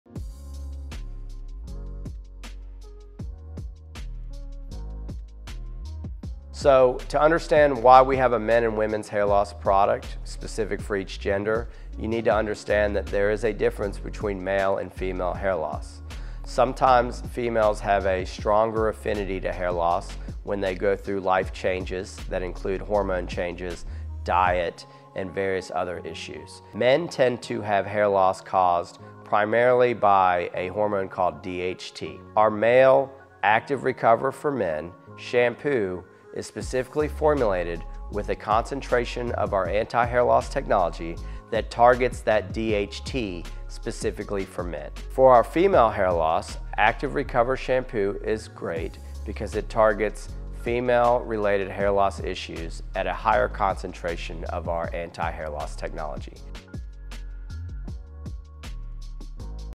Hair Loss Remedy | What Causes It and the Best Treatment | Men's vs Women's Shampoo
Microbiologist and Actiiv founder, Jared Reynolds, discusses the difference between men's and women's hair loss including what causes it and how to fix it.

Want to learn more? Follow Actiiv on social media to stay up to date!